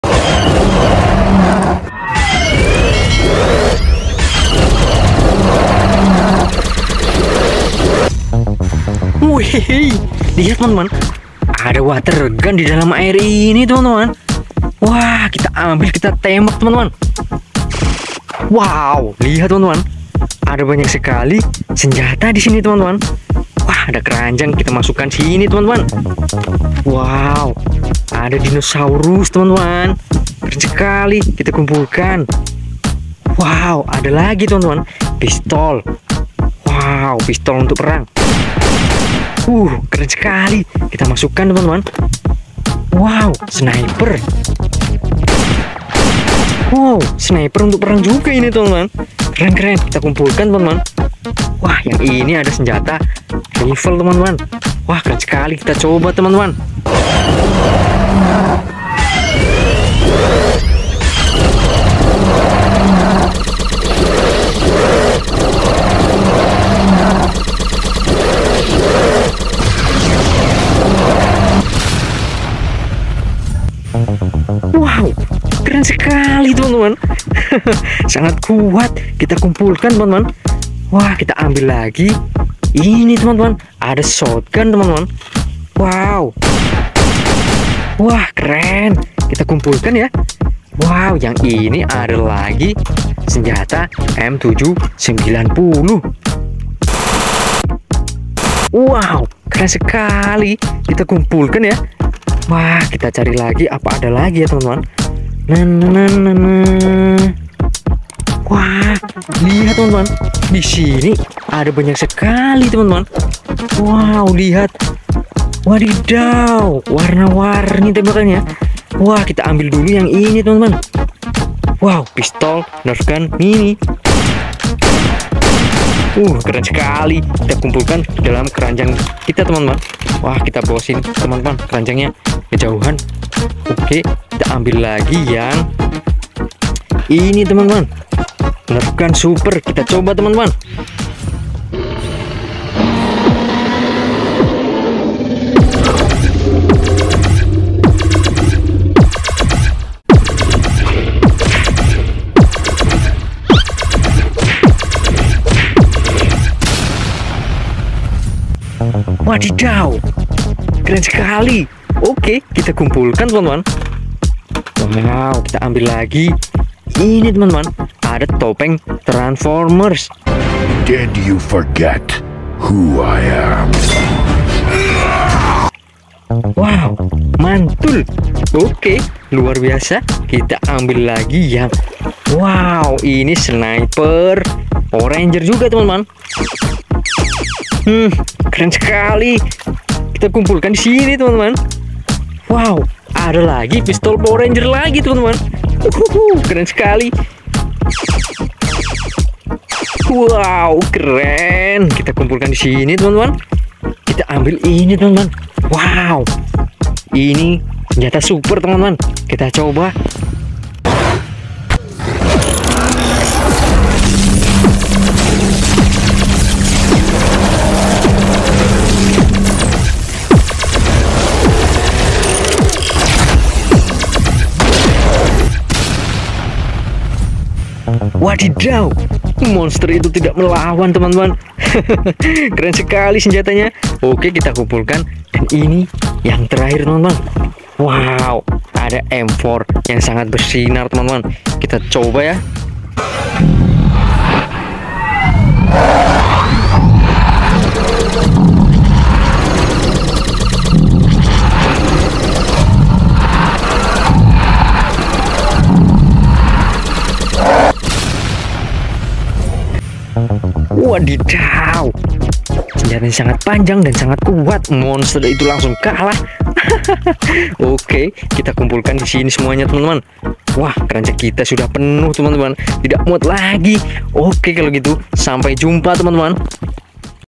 Collecting 7 Sniper Rifles and AK47 Guns, Pistol, AWM, Nerf Gun, M416 Gun, Shotgun
hello my friend, I found a super shot that can be used to defeat monsters, it's amazing, don't forget to subscribe and like this video, my friend, so that it can grow even more, thank you